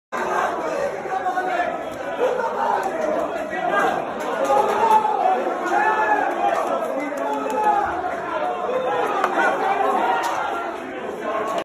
CALLERI emocionado tras eliminar a PALMEIRAS
La emoción de Jonathan Calleri y el abrazo con Rogério Ceni: San Pablo pasó por penales ante Palmeiras en la Copa de Brasil.